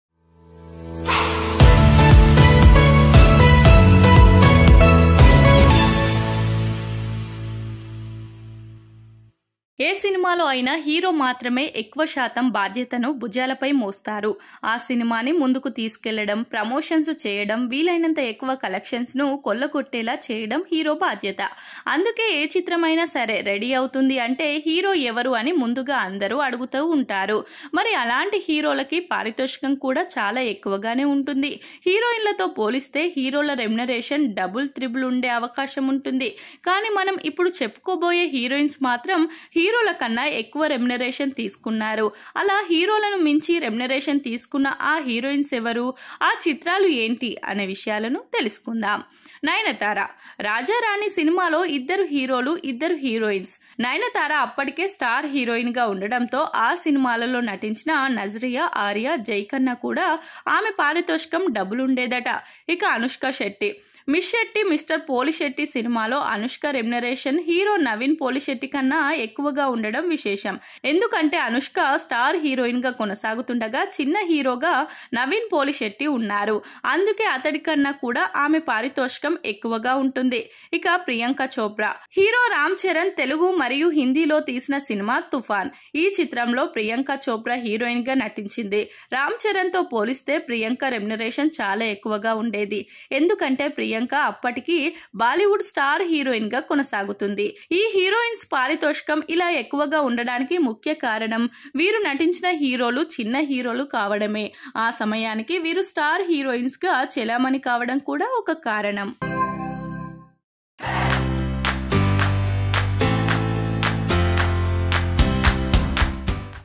హీరోల కన్నా హీరోయిన్స్ ఎక్కువ పారితోషకం తీసుకున్న సినిమాలు ఇవే ! heroines remuneration | FBTV NEWS
► About The Video :హీరోల కన్నా హీరోయిన్స్ ఎక్కువ పారితోషకం తీసుకున్న సినిమాలు ఇవే ! heroines remuneration | FBTV NEWS